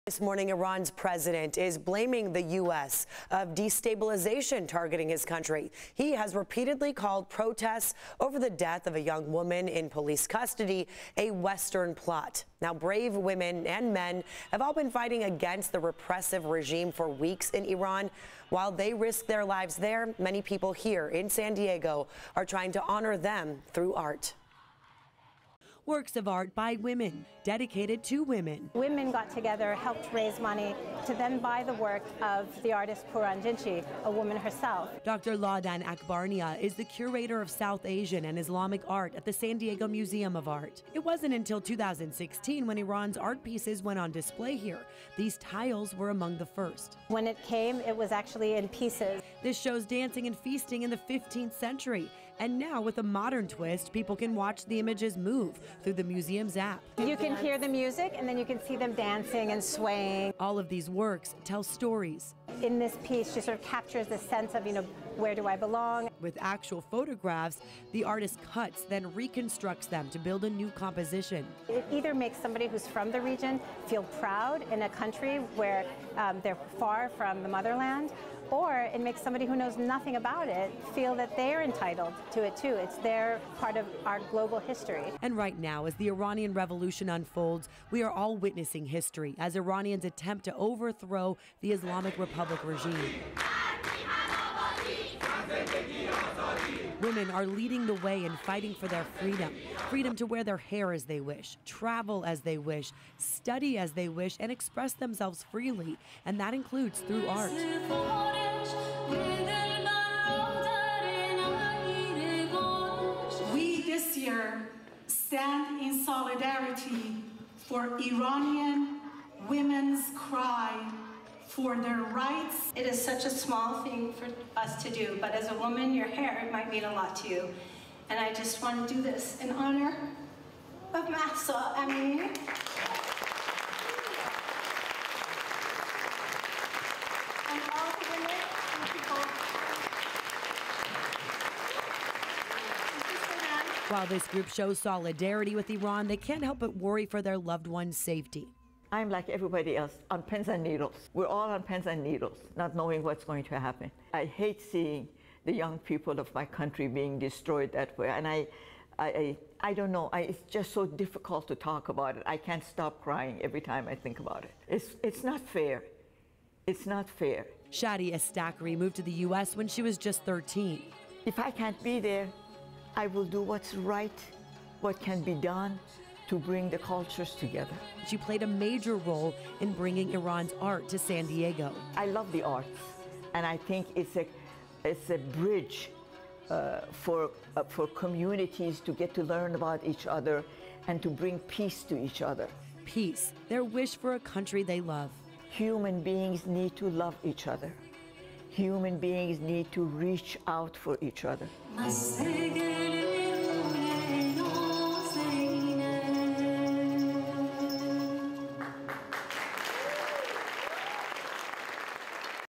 San Diegans dedicate art and music to women of Iran
The images we see of Iran lately are of brave women and men fighting against a repressive regime. While they risk their lives in Iran, many people here in San Diego are trying to honor them. CBS 8’s Neda Iranpour shows us how they’re doing so through art.

Works of art by women, dedicated to women.

“Women got together, helped raise money to buy the work of Pouran Jinchi, a women herself,” says Dr. Ladan Akbarnia who is the curator of South Asian and Islamic Art at the San Diego Museum of Art.